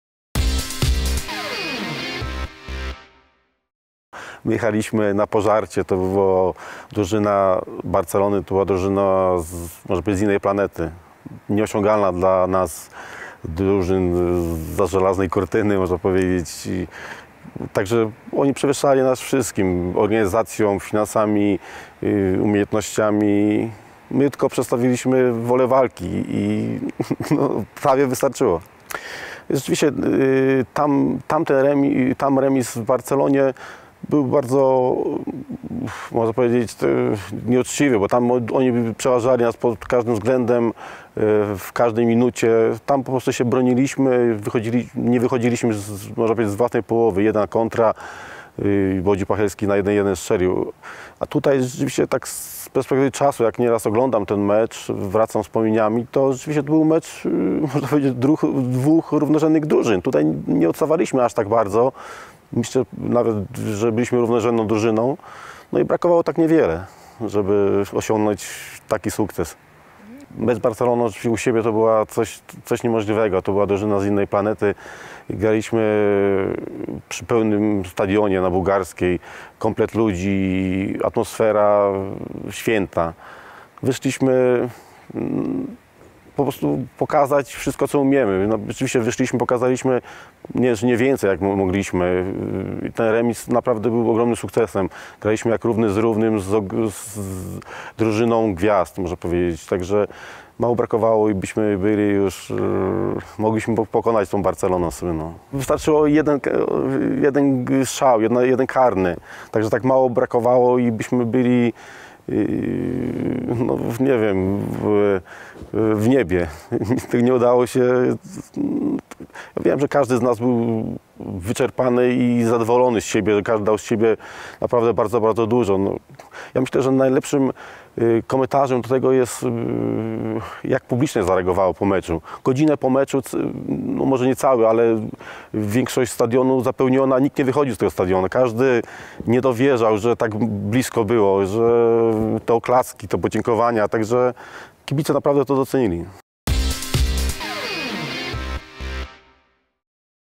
Marek Rzepka: To była drużyna z innej planety
Ćwierć wieku graliśmy jak równy z równym z wielką Barceloną. Było tak blisko awansu, wystarczył jeden wykorzystany karny - wspomina Marek Rzepka.